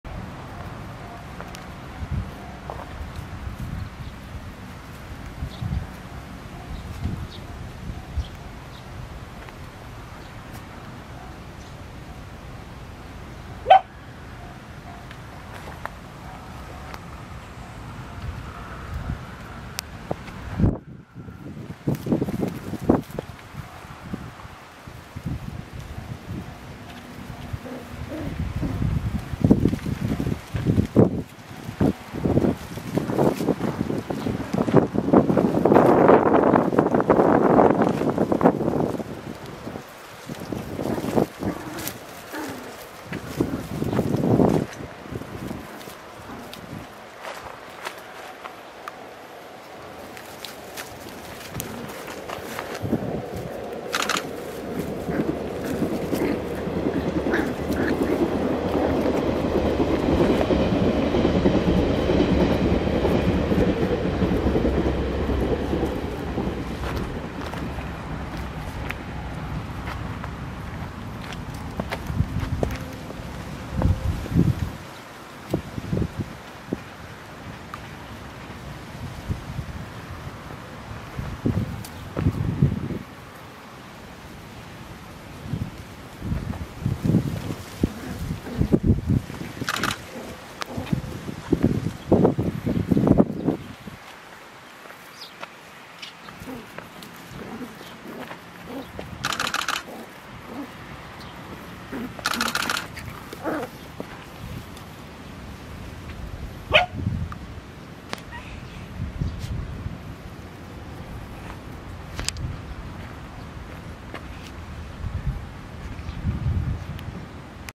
畑で遊ぶ子犬
楽しそうに畑で遊ぶもみじの子犬兄妹。
日に日に成長し行動範囲も広がってきました。